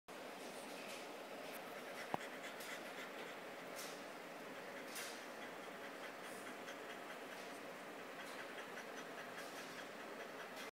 Dozer The French Bulldog cooling off
I found this video funny. After playing for awhile, Dozer posted up in front of the fan to cool off.